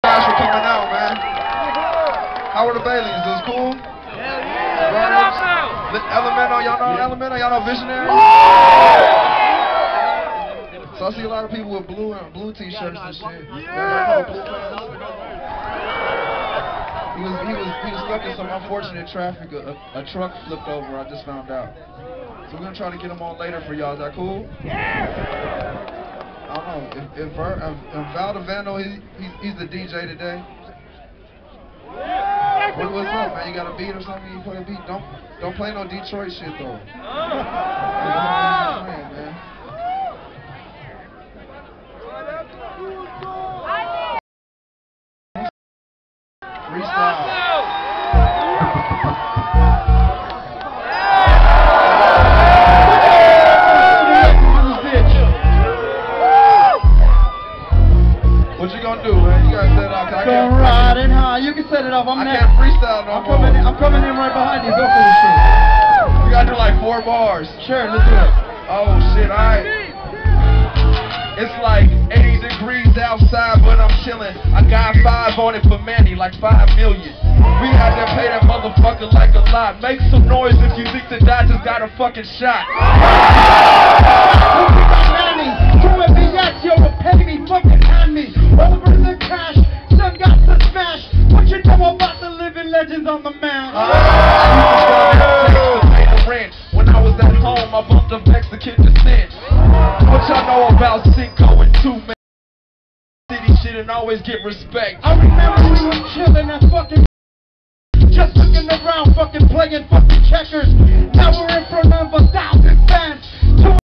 Murs- Freestyle Paid Dues 09
Murs and 2Mex Freestyle. Entire show wus dope!